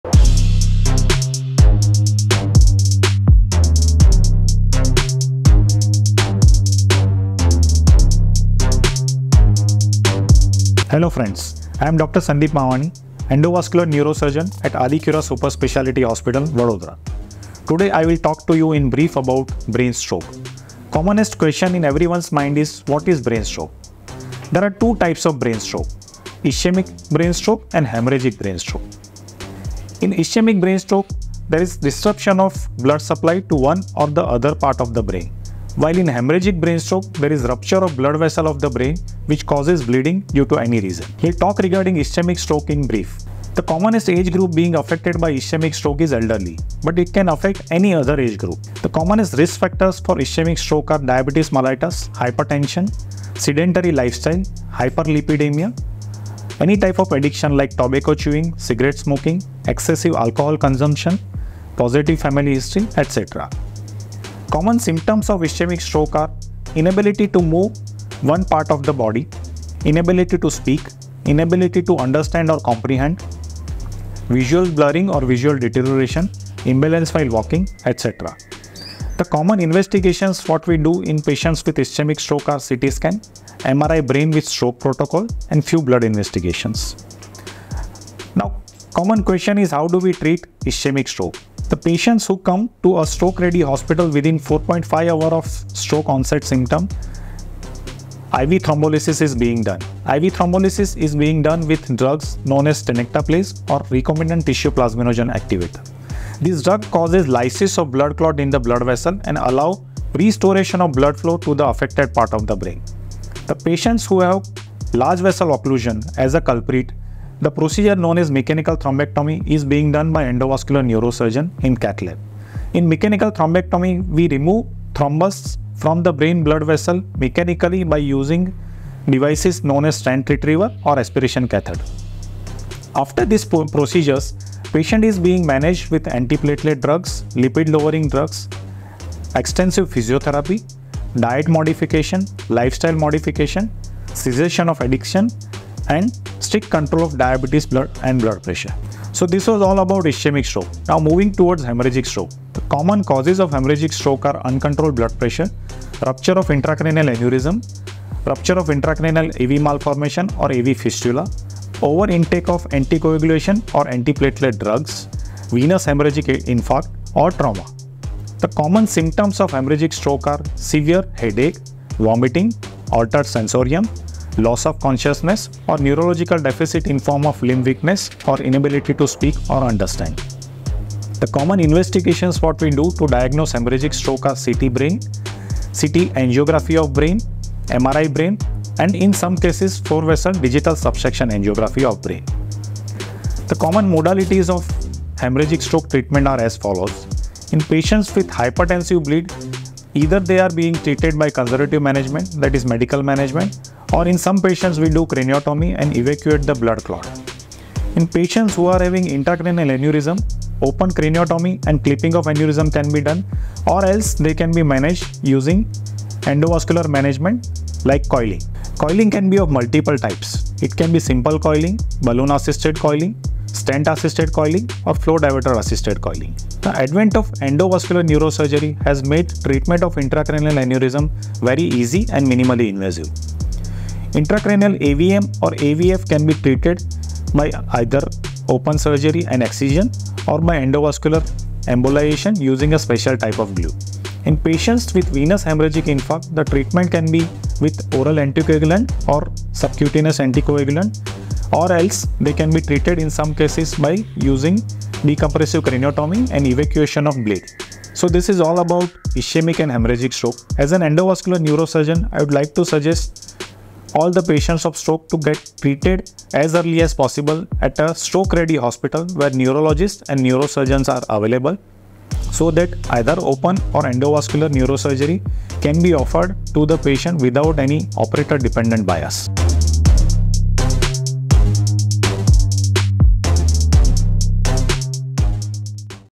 What is a brain stroke? | Dr Sandip Mavani | Aadicura Superspeciality Hospital
What to do if you have a brain stroke?|
Brain stroke is a very serious problem and if it is not diagnosed at the right time, it can definitely be fatal. But instead of panicking, it is important to understand its diagnosis. And in this video you will get to know the ways to get timely diagnosis and treatment of brain stroke.
If you liked the video then press the like button and if you find it useful then share it with your family and friends.